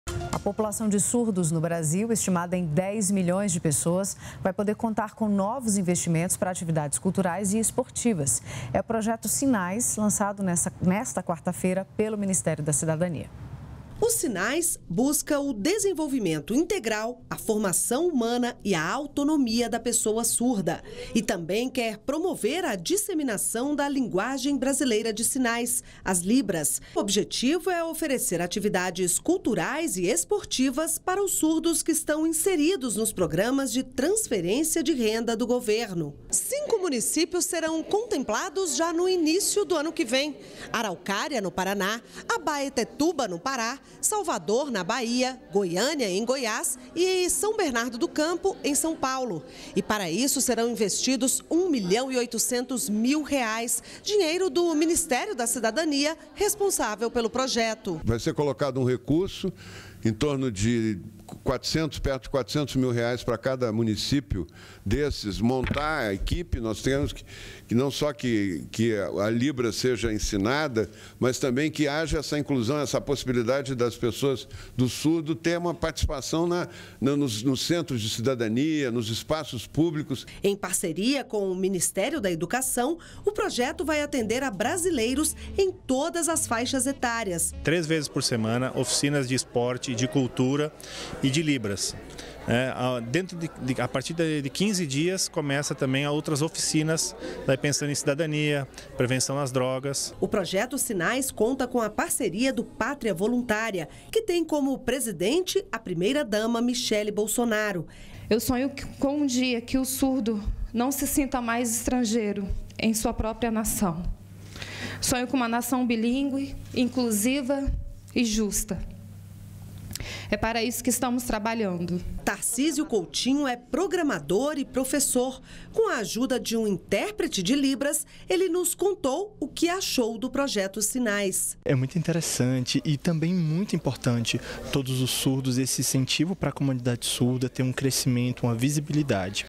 Projeto Sinais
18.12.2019 - Estimada em dez milhões de pessoas, a população de surdos no Brasil  contará com novos investimentos para atividades culturais e esportivas. É o Projeto Sinais, lançado hoje pelo Ministério da Cidadania.